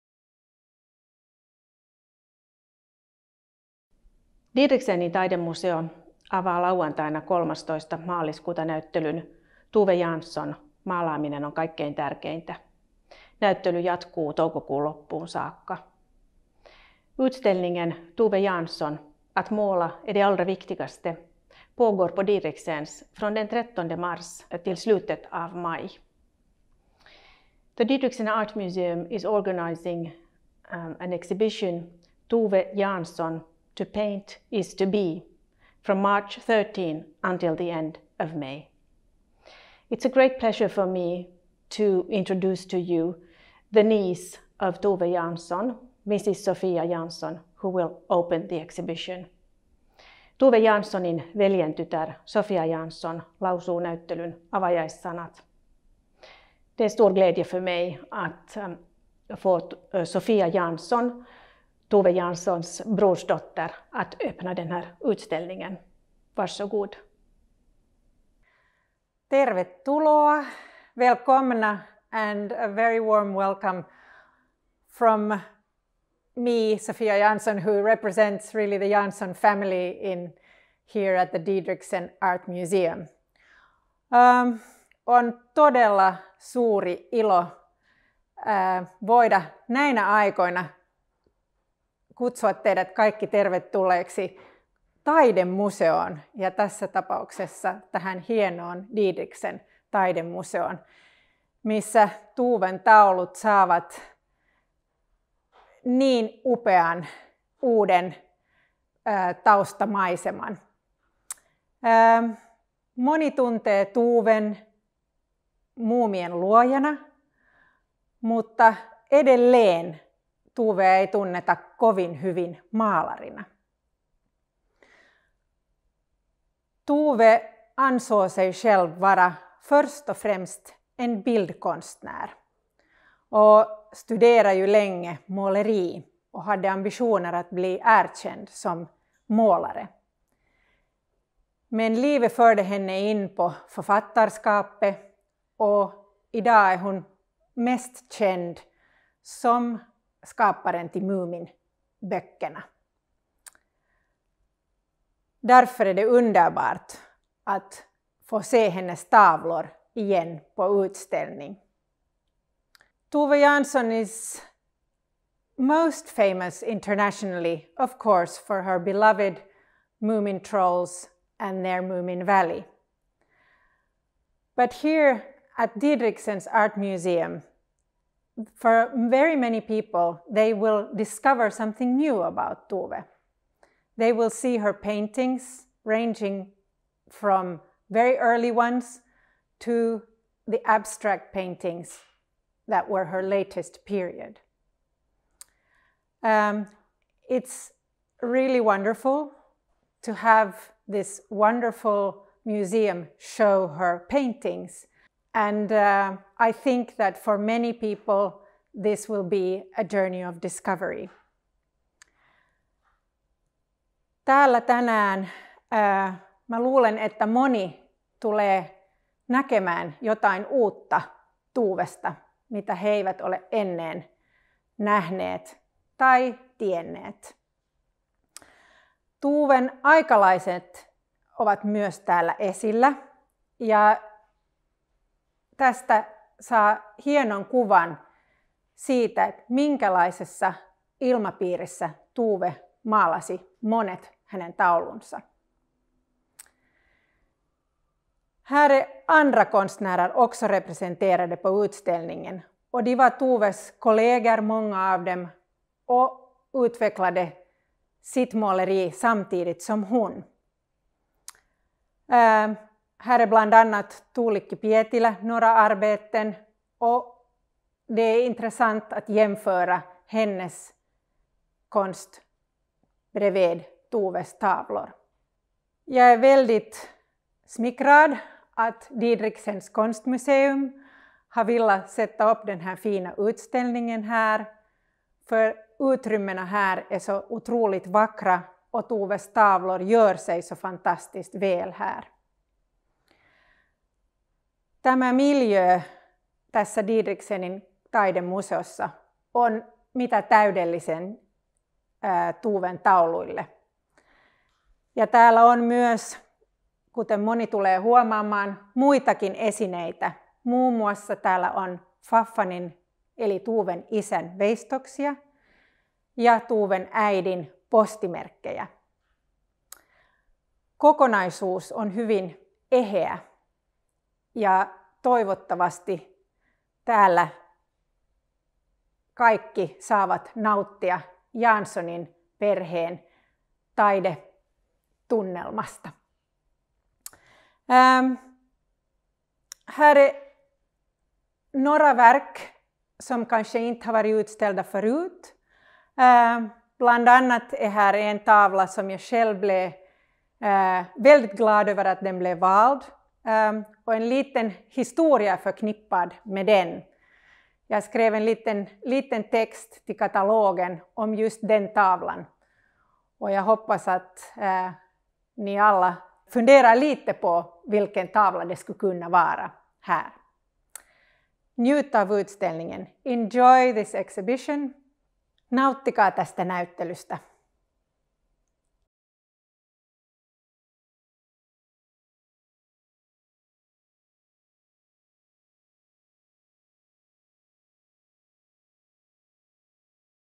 Tove Jansson - Opening of the exhibition
Tove Jansson – Maalaaminen on kaikkein tärkeintä -näyttely Didrichsenin taidemuseossa 13.3.–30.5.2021. Avajaispuheet: Maria Didrichsen ja Sophia Jansson.

Tove Jansson – To paint is to be, exhibition at the Didrichsen Art Museum 13.3.–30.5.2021. Opening speeches: Maria Didrichsen and Sophia Jansson.